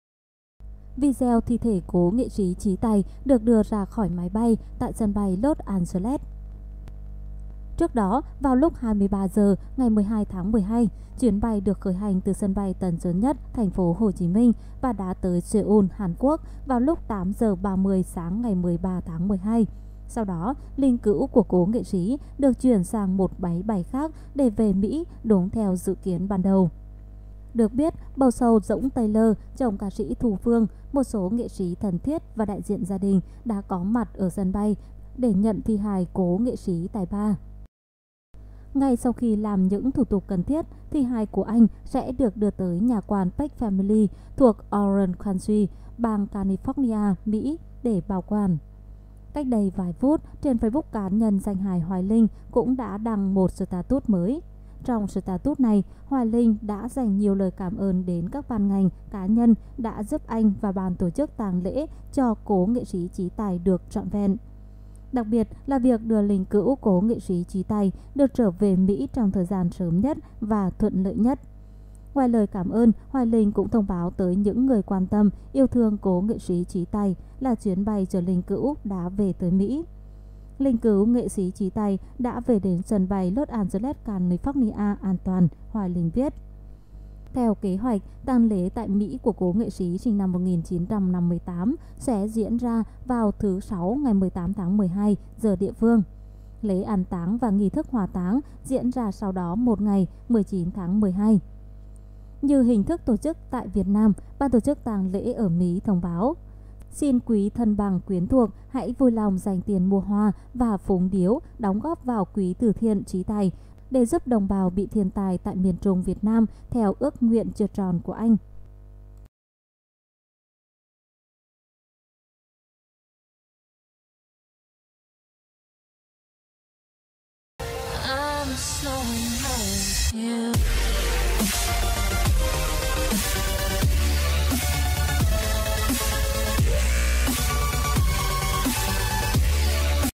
Vợ NS Chí Tài Có Vượt Qua Nổi Cú "Sốc" Khi Chồng Ra Đi Bất Ngờ ?
là Kênh tổng hợp các tin tức, sự kiện nổi bật trên tất cả các lĩnh vực trong ngày. Cập nhật nhanh và liên tục.